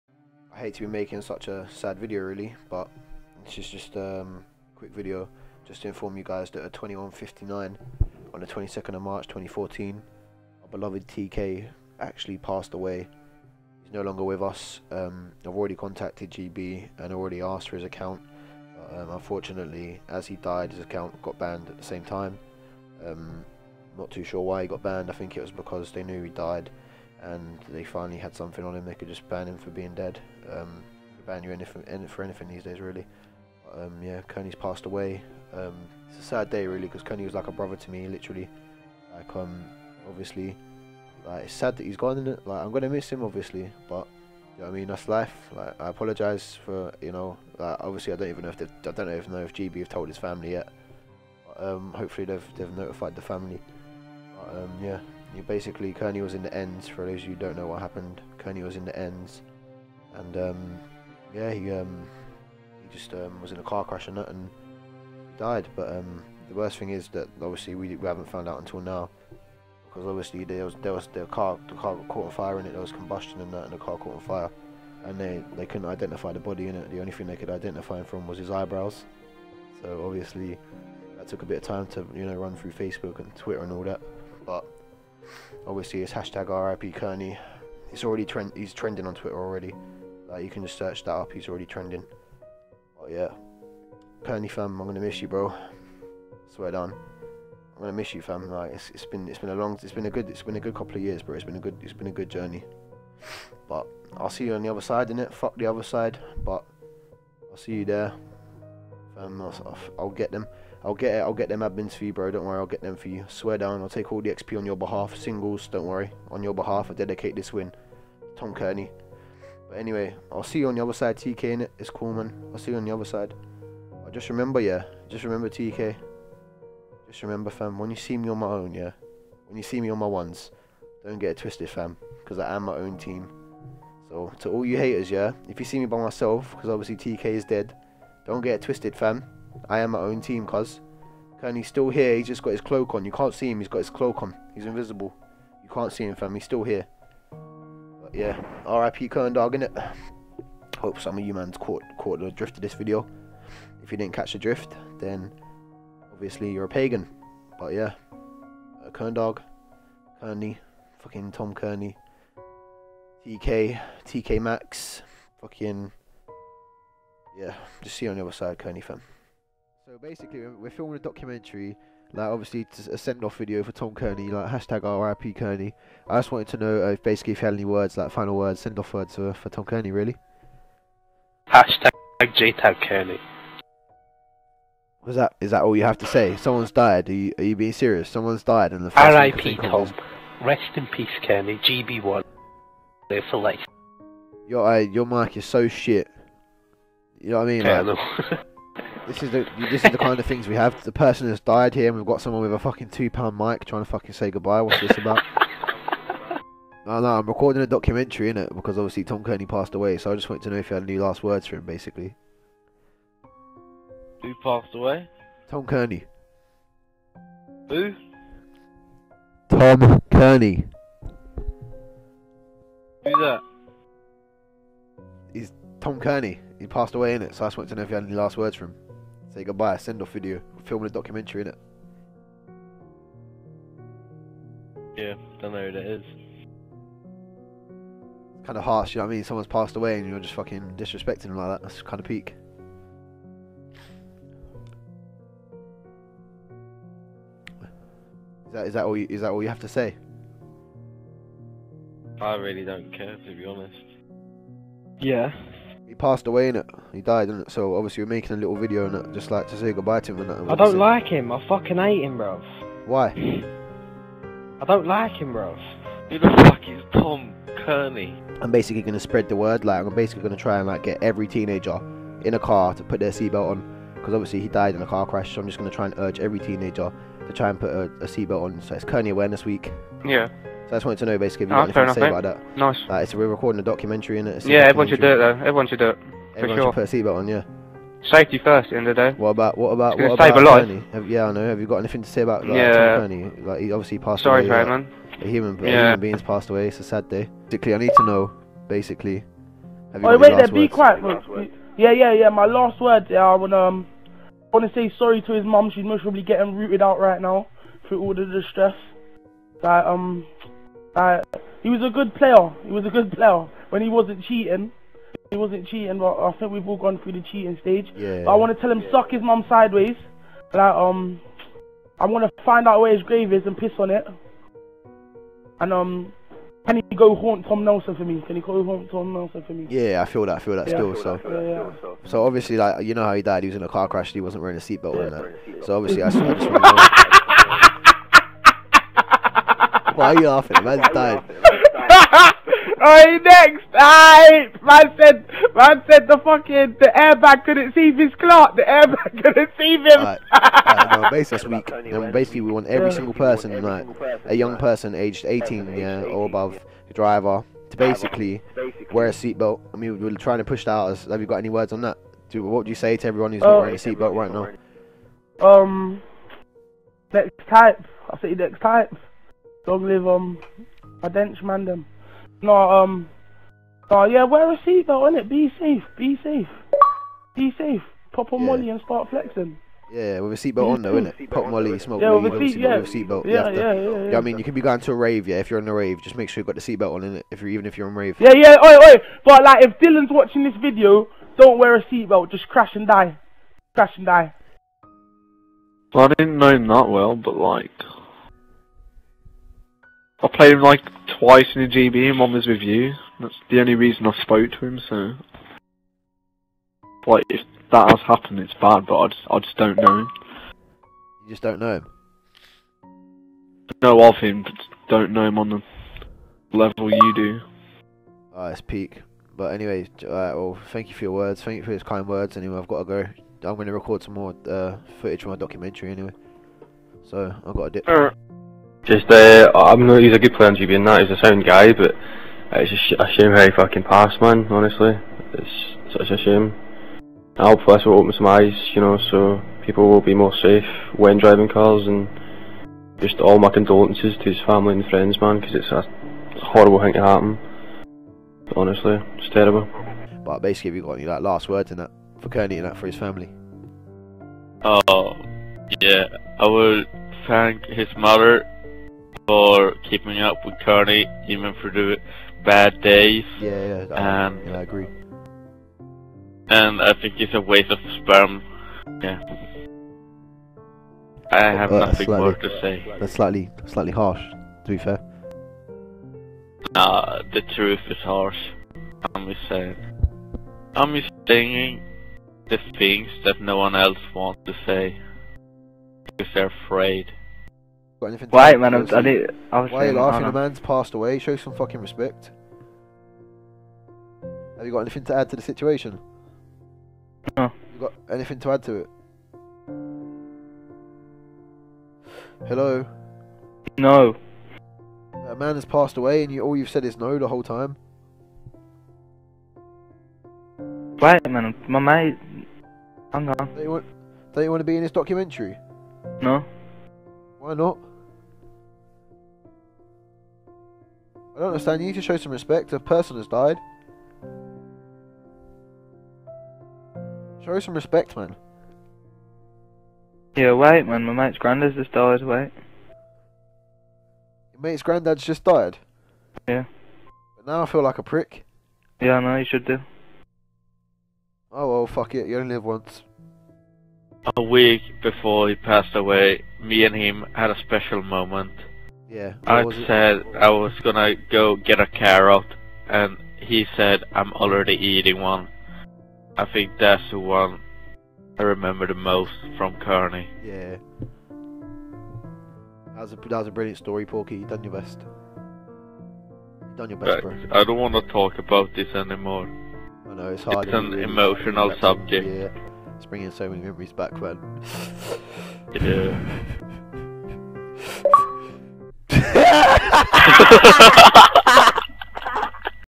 KearneyAwarenessWeek - RIP KEARNEY
RIP TOM KEARNEY. PLEASE SPREAD THE WORD ABOUT KEARNEY AWARENESS WEEK. Don't be a prick. Wear a seat belt.